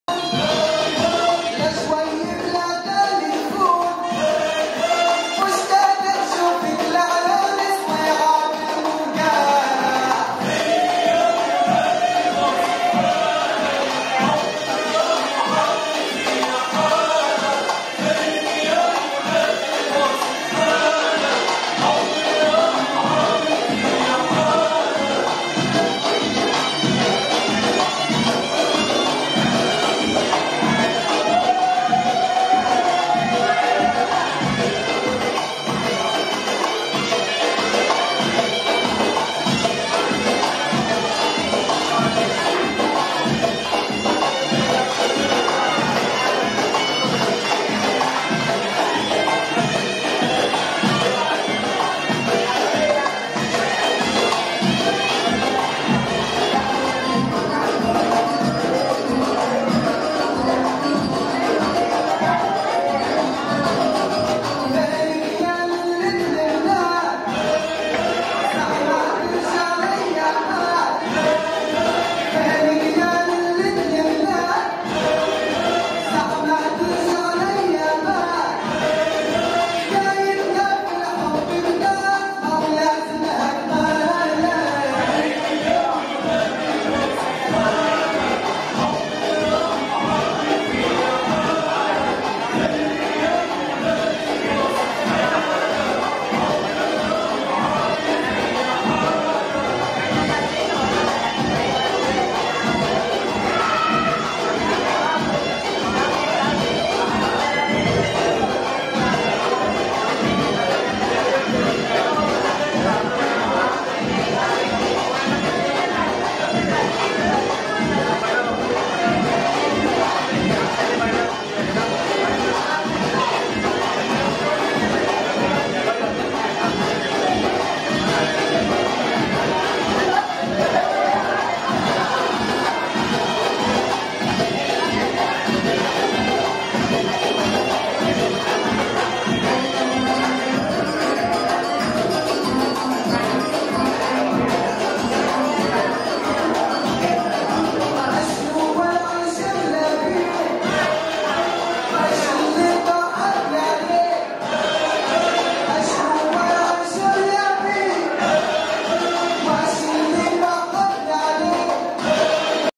Rosanna Valera performing arab folclore from the maghreb area. Club LSP
Bailando esta colorida y hermosa pieza de folclore, con e jarrón estilo magreb. (Raks Al Juzur Año 2016 en el Restaurante El Cedro Video gracias a Rudolf S.  ¡¡Cuánta energía!!